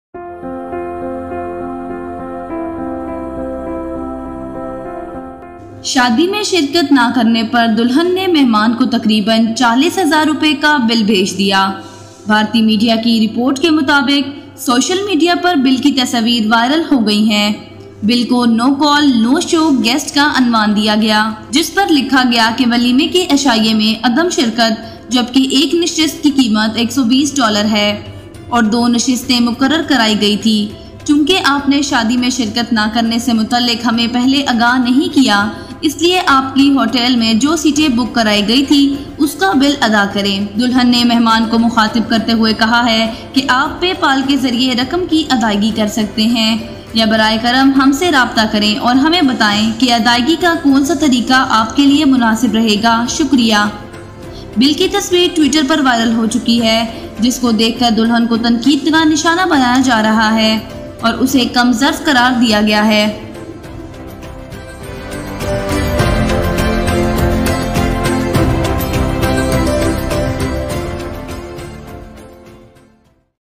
Shaadi Mai Sherkat Na Karny Par Dolhan Ny Mehman Ko Taqreban 40 Hazar Rupies Ka Bill Bhaij Deya
شادی میں شرکت نہ کرنے پر دلہن نے مہمان کو تقریباً 40 ہزار روپے کا بل بھیج دیا۔

بھارتی میڈیا کی رپورٹ کے مطابق سوشل میڈیا پر بل کی تصویر وائرل ہو گئی ہے، بل کو ’ نو کال، نو شو گیسٹ‘ کا عنوان دیا گیا جس پر لکھا گیا کہ ولیمے کے عشائیے میں عدم شرکت، جب کہ ایک نشست کی قیمت 120 ڈالر ہے اور دو نشستیں مقرر کرائی گئی تھیں۔ چونکہ آپ نے شادی شرکت نہ کرنے سے متعلق ہمیں پہلے آگاہ نہیں کیا اس لیے آپ کی ہوٹل میں جو سیٹیں بک کرائی گئی تھیں اس کا بل ادا کریں۔

دلہن نے مہمان کو مخاطب کرتے ہوئے کہا ہے آپ PayPal کے ذریعے رقم کی ادائیگی کر سکتے ہیں یا براہ کرم ہم سے رابطہ کریں اور ہمیں بتائیں کہ ادائیگی کا کون سا طریقہ آپ کے لئے مناسب رہے گا۔ شکریہ!”

بل کی تصویر ٹوئٹر پر وائرل ہو چکی ہے جس کو دیکھ کر دلہن کو تنقید کا نشانہ بنایا جا رہا ہے اور اسے کم ظرف قرار دیا گیا ہے۔